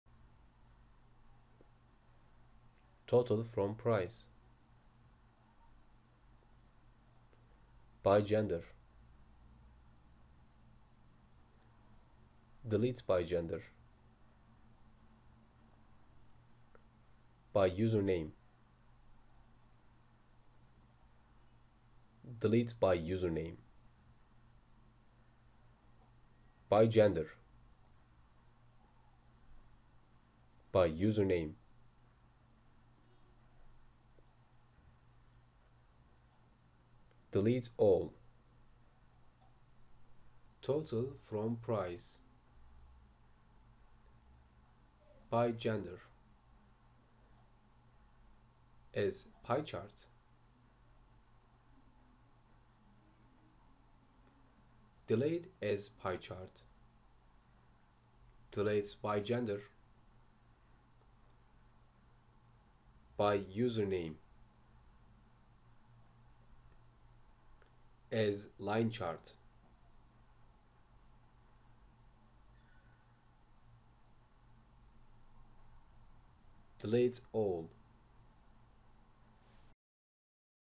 Power BI for Office 365 (Q&A) Üzerinde Ses Komutları ile Rapor Oluşturmak
Küçük bir test.

Windows Speech Recognition kullanarak Office 365 Power BI Site   üzerinde ses komutları ile Power View (Q&A) raporları oluşturuyoruz.